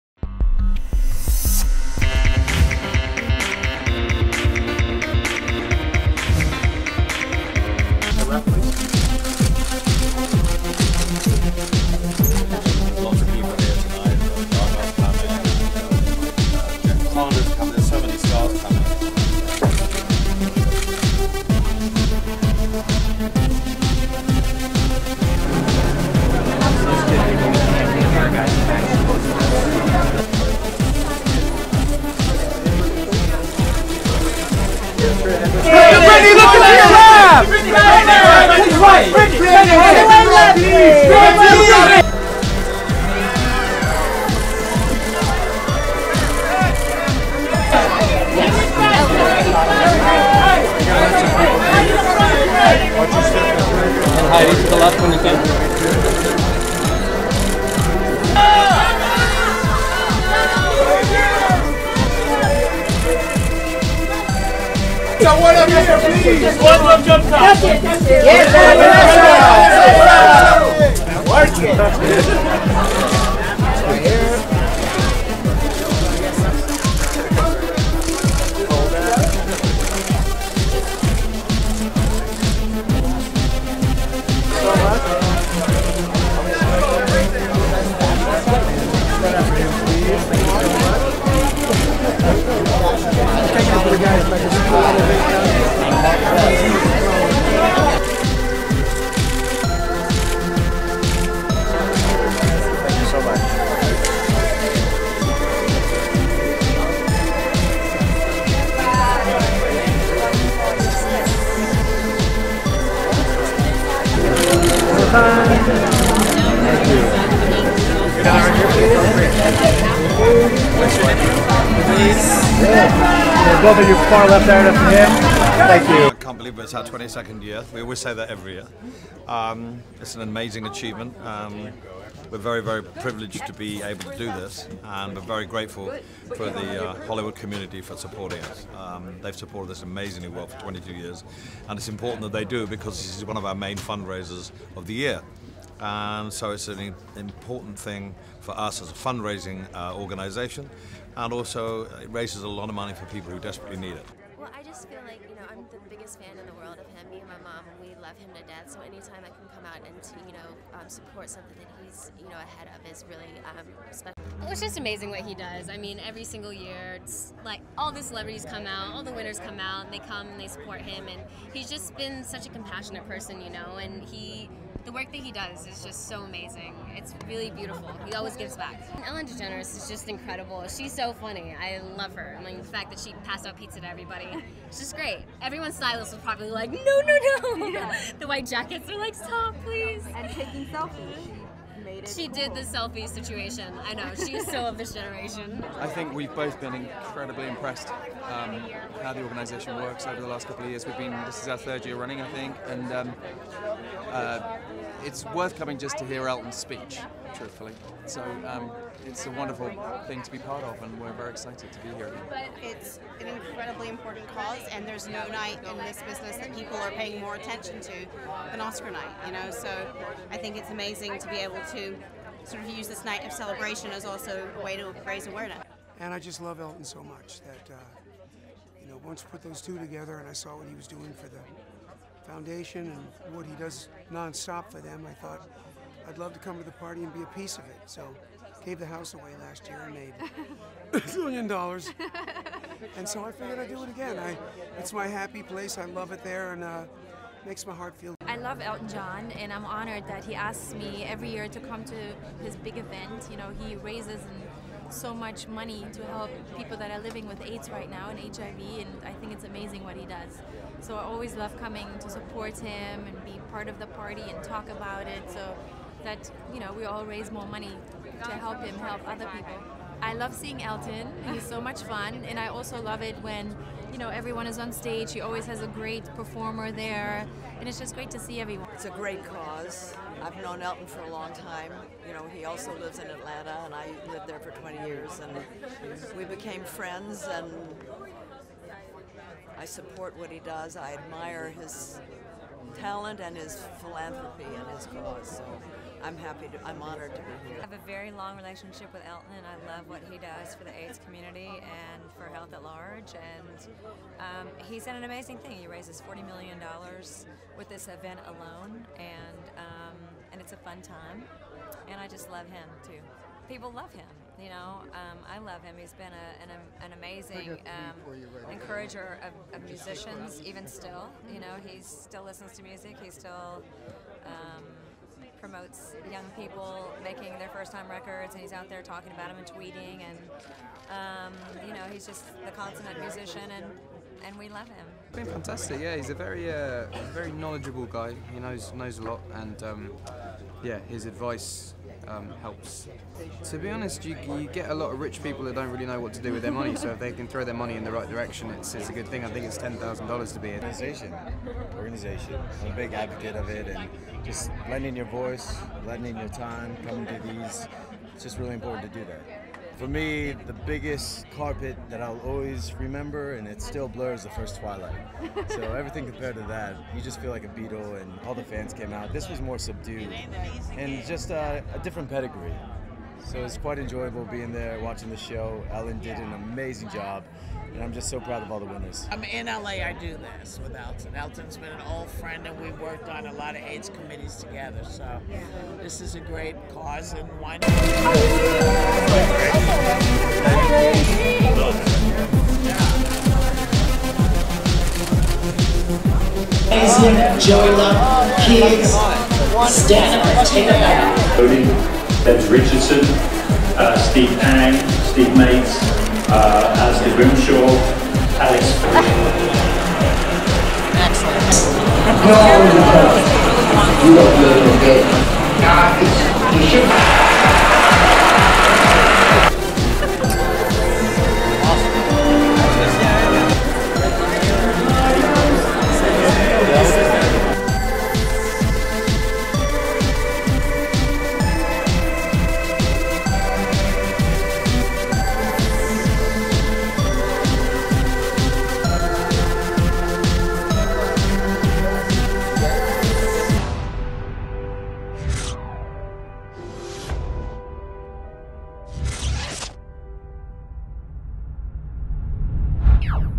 Elton John AIDS Foundation's Oscar Party, Britney Spears, Vanessa Hudgens, Lady Gaga
22nd Annual Elton John AIDS Foundation's Oscar Viewing Party - Celebrity Arrivals March 02, 2014 - West Hollywood, California, United States